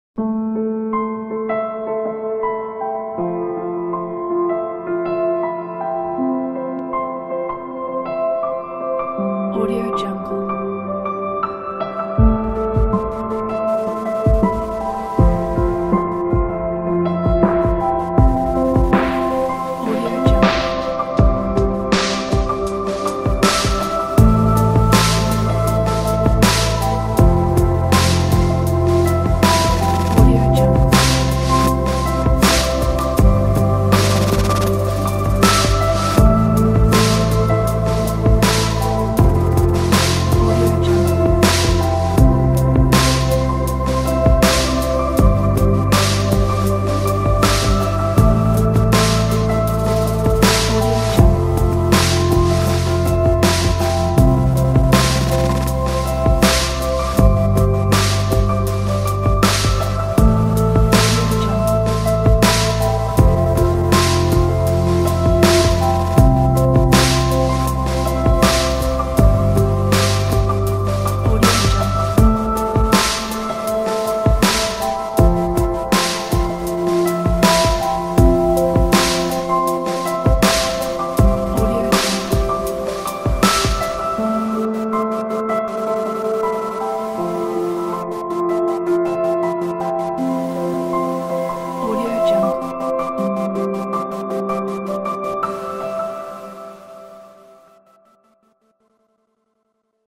After Effects Project Files - On The Run - A Travel Slideshow - VideoHive 9520792
FEATURES: Modular structure: use a big variety of layouts to build your own sequence 12 scenes, 20 placeholders Full HD No 3rd party plug-ins required Color scheme controls Video tutorial included... (openers) Read more